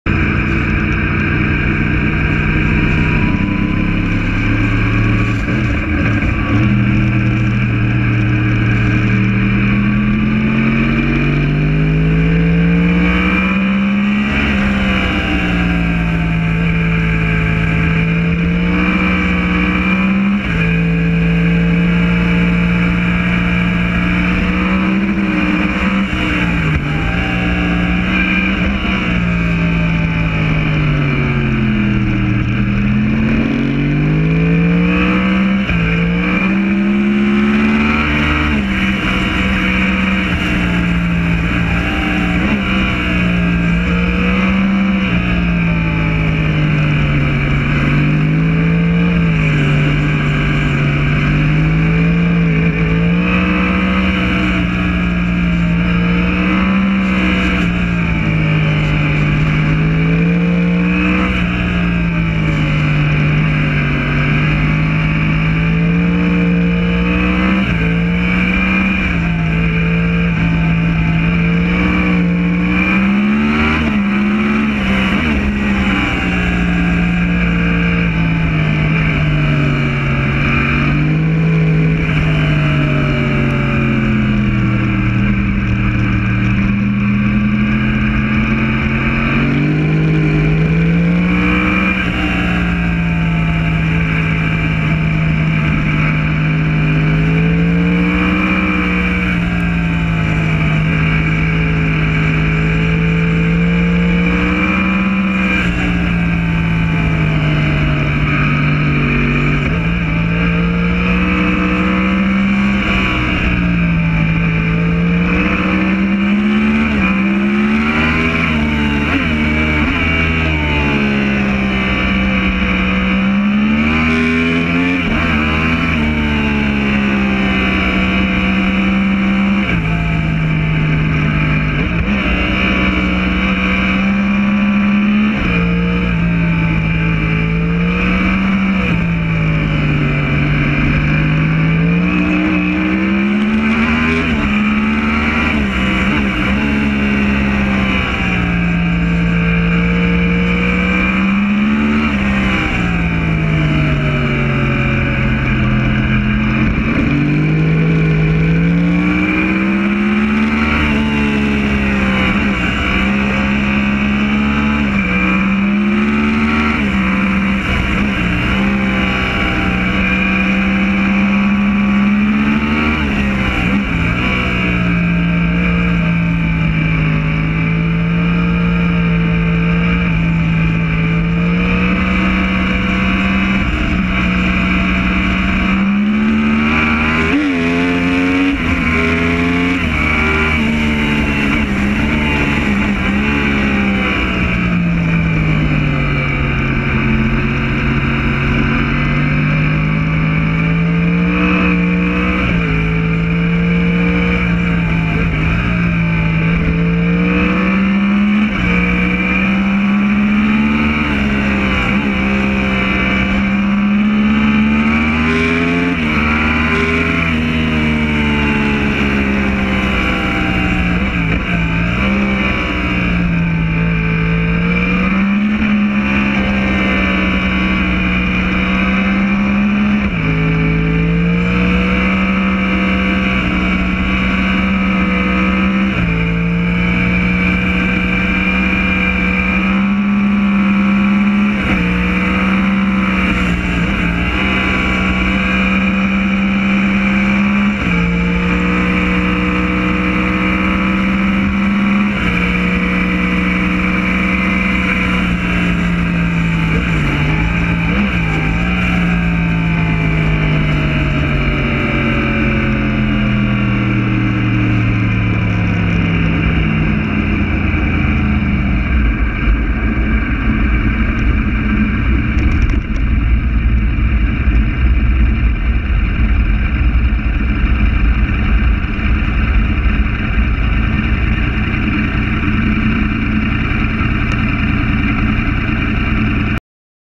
Yamaha R1 Riding to Franschhoek on R45 Pass. RAW
No Music. No edit. just plain full video! Epic